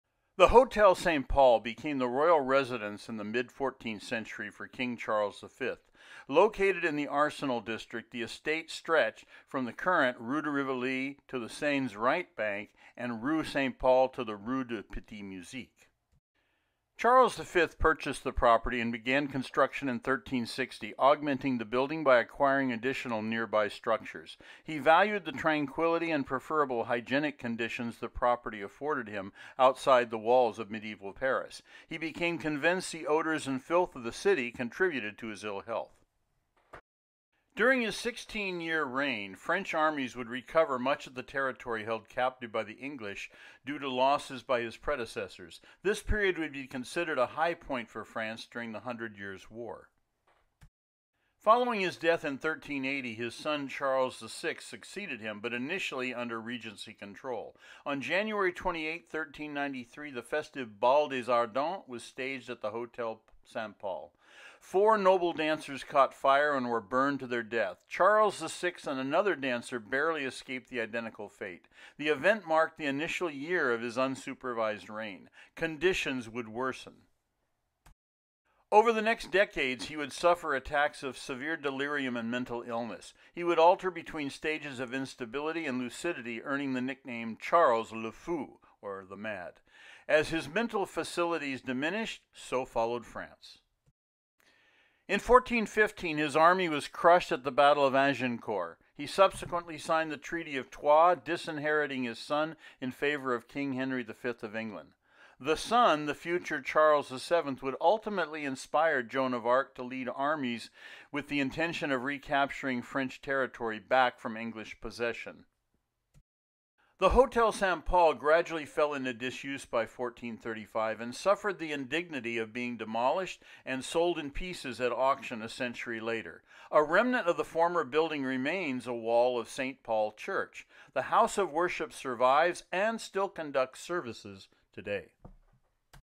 St. Paul Church and An Insane French King
The Hotel Saint-Pol became the royal residence in the mid 14th century for King Charles V. Located in the Arsenal district, the estate stretched from the current rue de Rivoli to the Seine’s Right Bank and rue Saint Paul to the rue du Petit-Music. Charles V purchased the property and began construction in 1360 augmenting the building by acquiring additional nearby structures. He valued the tranquility and preferable hygienic conditions the property afforded him outside the walls of medieval Paris. He became convinced the odors and filth of the city contributed to his ill health.

During his sixteen-year reign, French armies would recover much of the territory held captive by the English due to losses by his predecessors. This period would be considered a high point for France during the Hundred Years’ War.

Following his death in 1380, his son Charles VI succeeded him, but initially under regency control. On January 28, 1393, the festive Bal des Ardents was staged at the Hotel Saint-Pol. Four noble dancers caught fire and were burned to their death. Charles VI and another dancer barely escaped the identical fate. The event marked the initial year of his unsupervised reign. Conditions would worsen.

Over the next decades, he would suffer attacks of severe delirium and mental illness. He would alter between stages of instability and lucidity earning the nickname Charles Le Fou (The Mad). As his mental capacities diminished, so followed France.

In 1415, his army was crushed at the battle of Agincourt. He subsequently signed the Treaty of Troyes disinheriting his son in favor of King Henry V of England. The son, the future Charles VII would ultimately inspire Joan of Arc to lead armies with the intention of recapturing France territory back from English possession.

The Hotel Saint-Pol gradually fell into disuse by 1435 and suffered the indignity of being demolished and sold in pieces at auction a century later. A remnant of the former building remains a wall of Saint-Paul Church. The house of worship survives and still conducts services today.